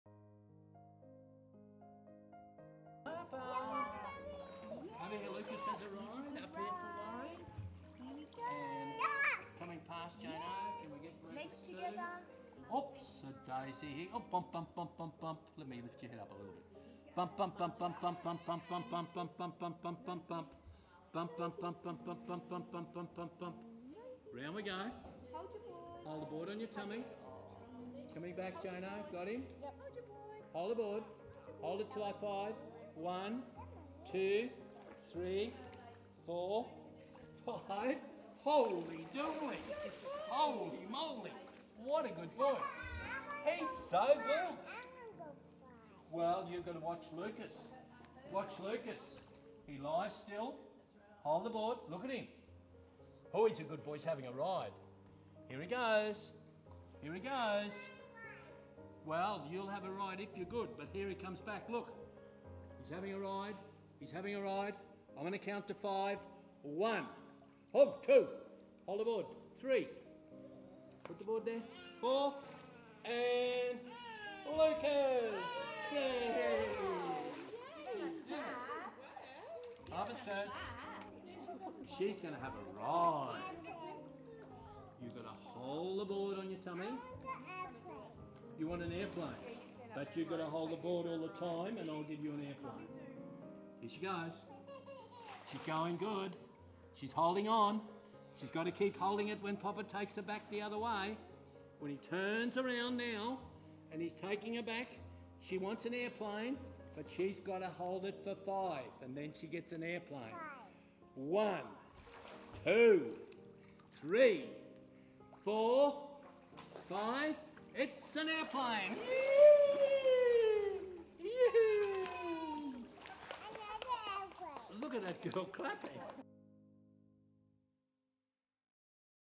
Back float for toddlers
Here's a fun way to encourage back floating in toddler swimming. Get more online swim teaching progressions:
Swim Professional Development academy - get all of Laurie's content in one location